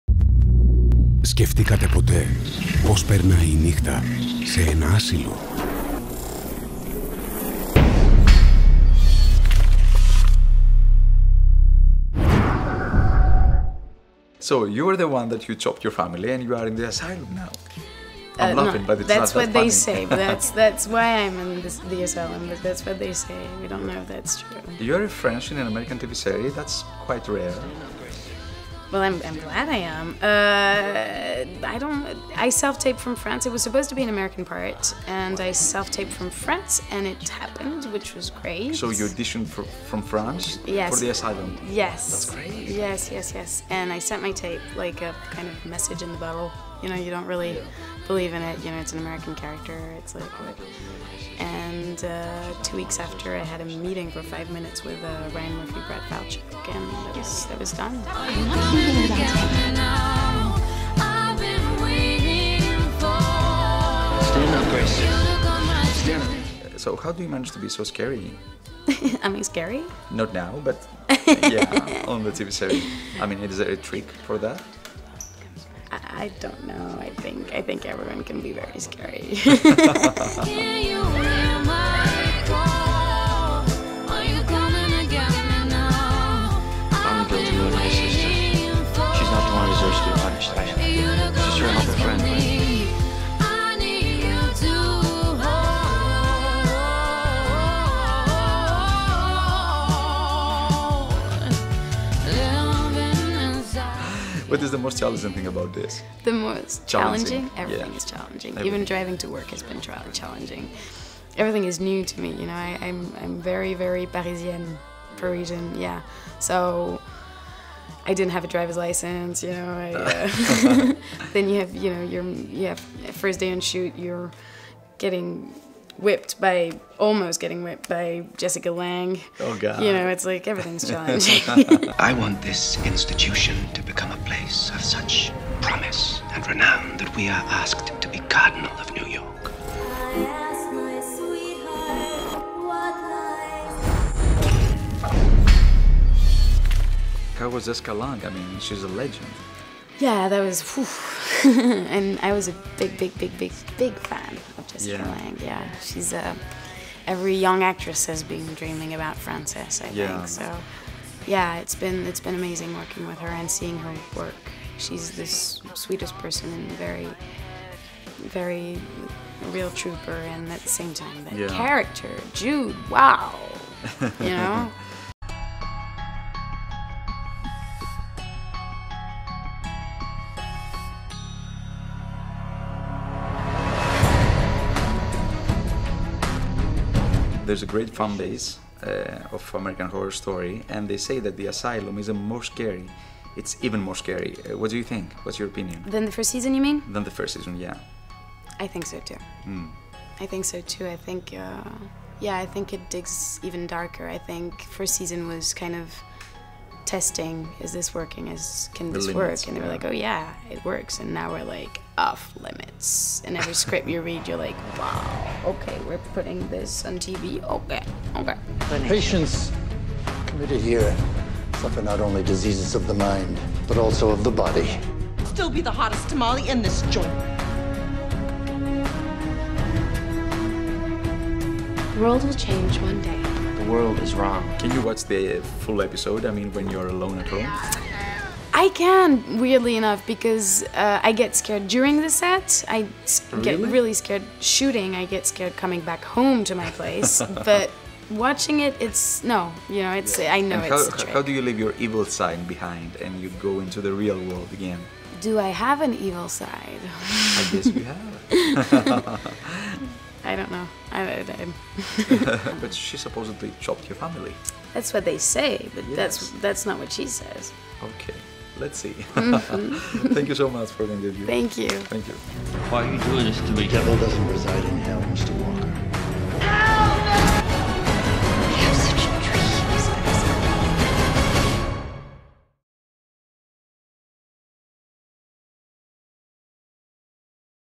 Lizzie Brochere - Interview with "American horror story's" french star in LA for MTV Greece.
George Satsidis interviews Lizzie Brochere on the set of American horror story: Asylum at Paramaount studios in L.A